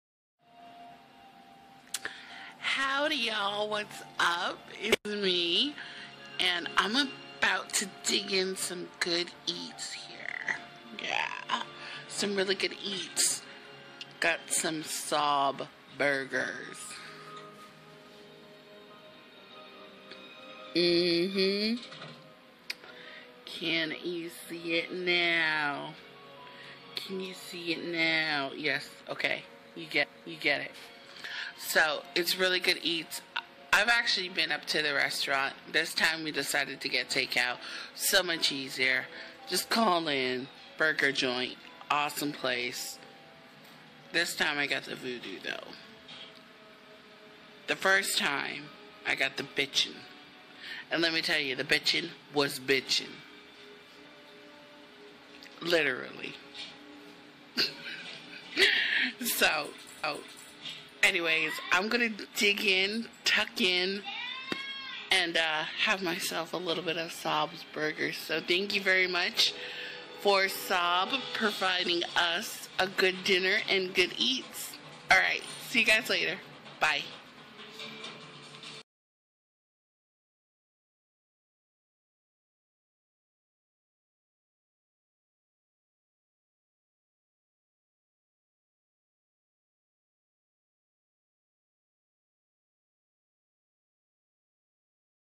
Awesome Burger! The Vodoo from SOB Burgers
Hubby surprised me again! SOB Burgers! The biggest burgers in town!!! I couldn't thank him enough for this!..

***BEST BURGER JOINT on this side of Bradenton
SOB Burgers
5866 14th ST W
sobburgers.net
Sorry this wasn't a Mukbang didn't have enough time to set up yet

help me get my youtube up and running ..give me some ideas..